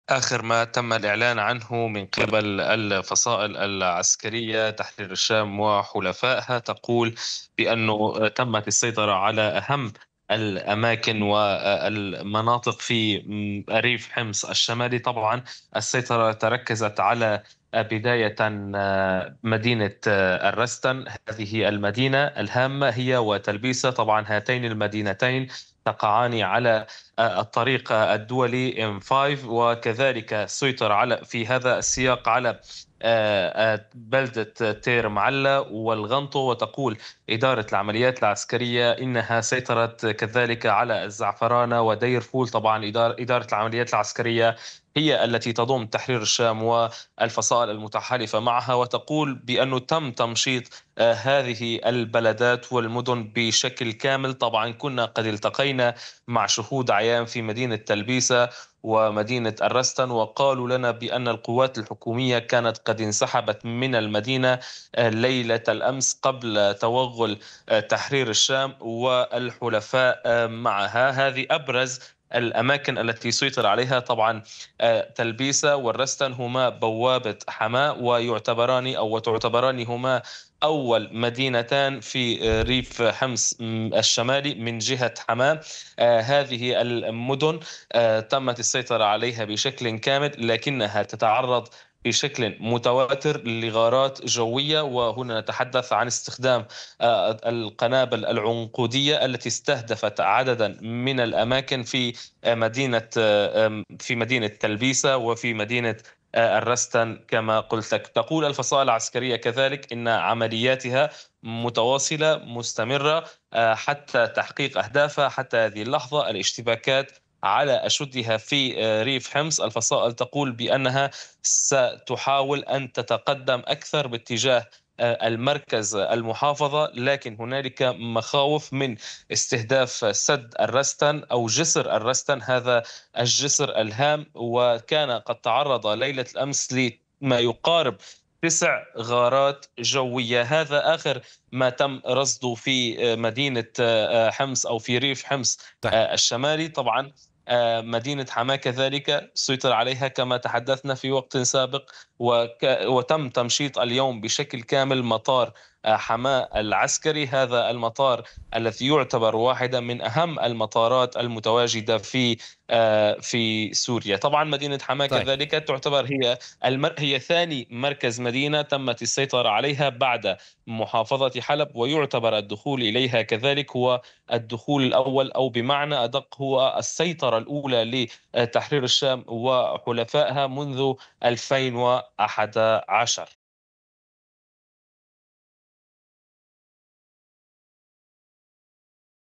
غارات وقصف بالقنابل العنقودية على تجمعات الفصائل شمال حمص
مراسل الحدث محمد هارون: غارات وقصف بالقنابل العنقودية على تجمعات الفصائل المسلحة في الرستن وتلبيسة بريف حمص الشمالي